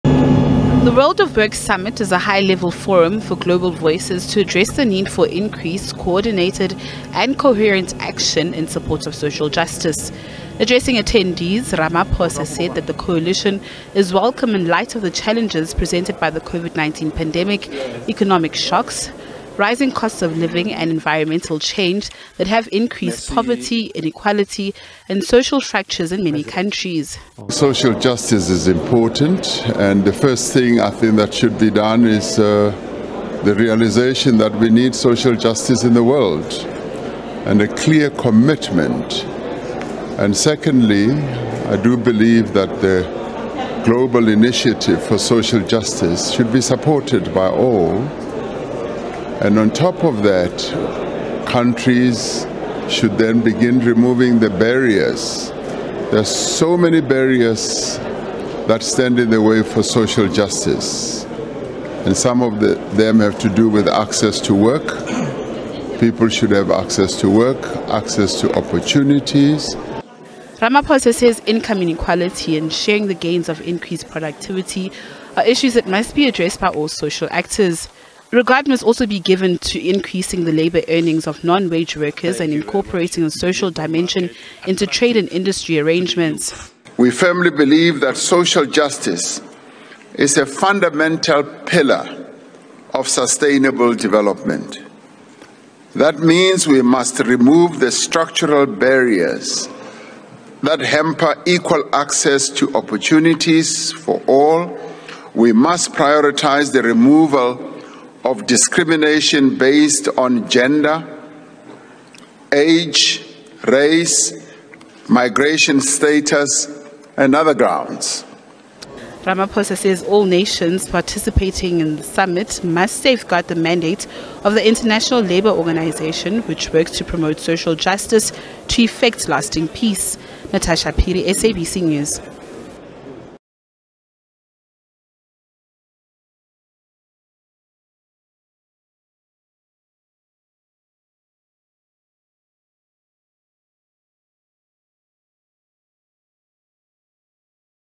Ramaphosa endorses ILO's call for global coalition for social justice
President Cyril Ramaphosa has on behalf of South Africa, endorsed a proposal to establish a global coalition for social justice.
The President has addressed dignitaries at the World of Work Summit in Geneva, Switzerland.
The gathering, hosted by the International Labour Organisation, is themed, 'Social Justice for All'.
Ramaphosa, as Co-Chair of the ILO's Global Commission on the Future World of Work, presented key recommendations of the Commission to attendees.